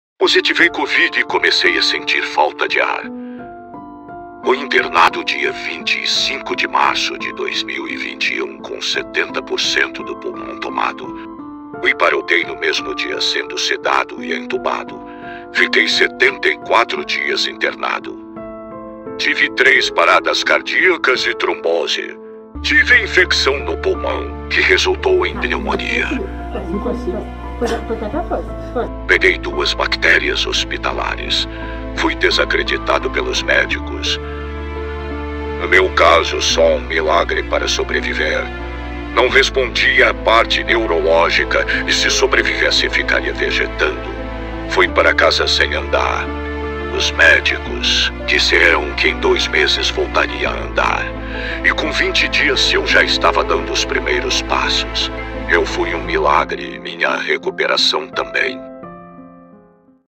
EU SOU UM MILAGRE | Tiago José - o ciclista que venceu a COVID-19
Tiago José também confirmou a sua participação no 03° Desfio De Marchi de MTB. Mas antes de postar a sua foto oficial, nos dê apenas 1️⃣ minuto para mostrar um pequeno resumo, disso que podemos chamar de MILAGRE